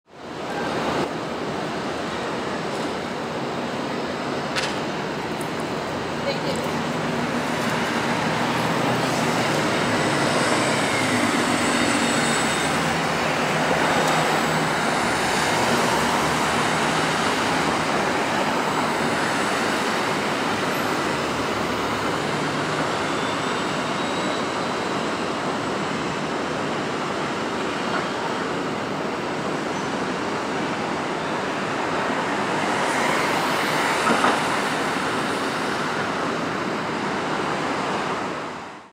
Charm City Circulator Orion VII 3G #1202 on the Purple Route
Penn Station bound.